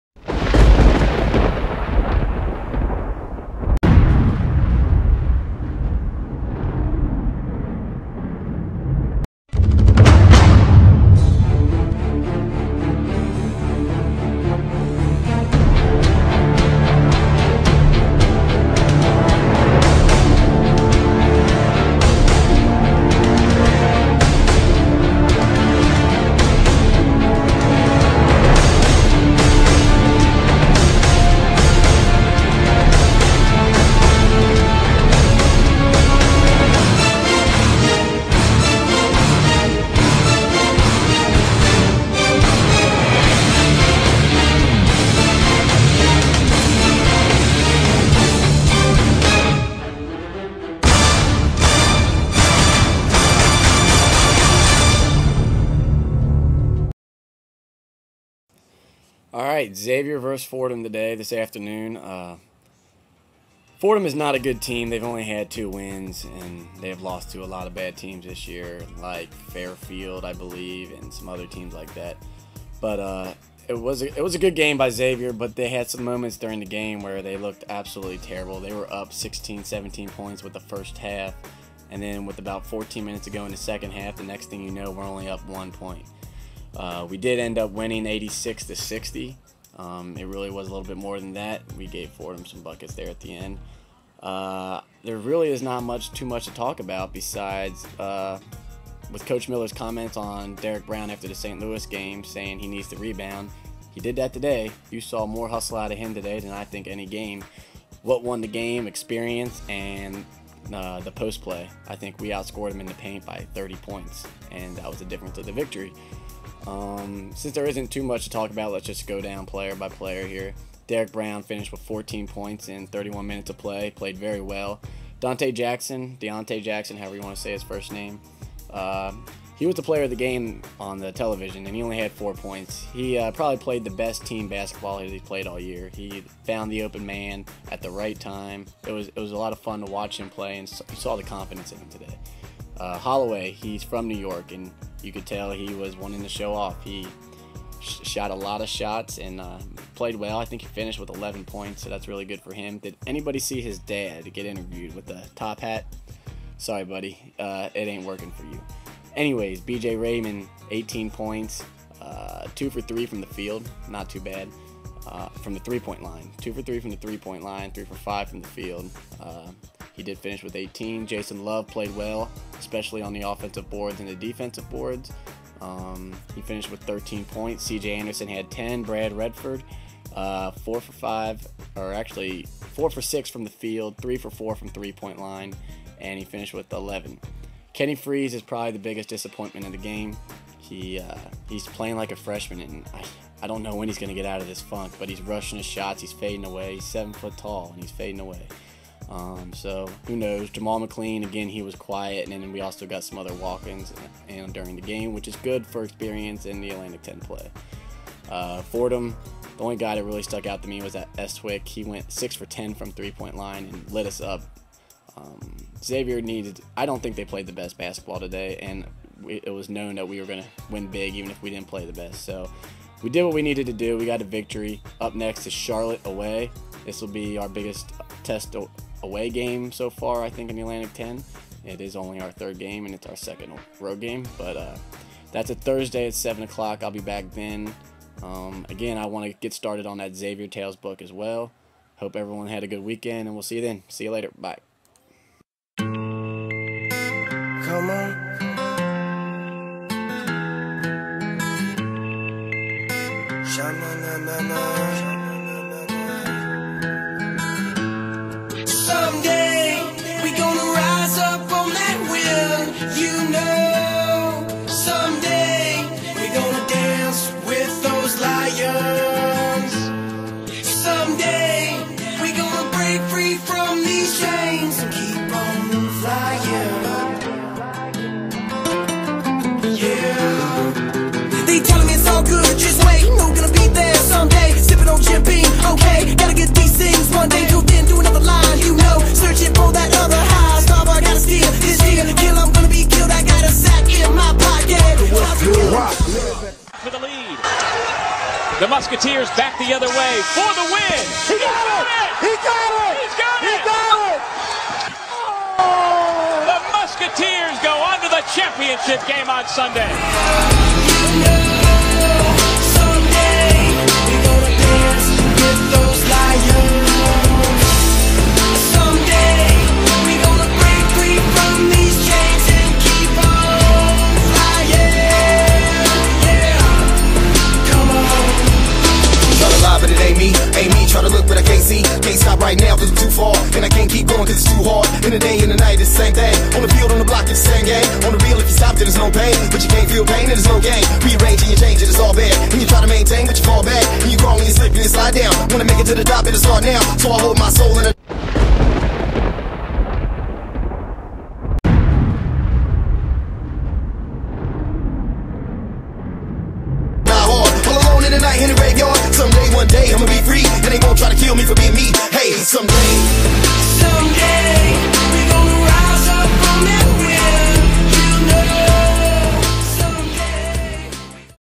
XU vs Fordham
his video was tough to make but here is the features:

1. Xavier Intro Video

2. Fordham analysis

3. Some D Brown and other highlights

4. Music and pics as always. ENJOY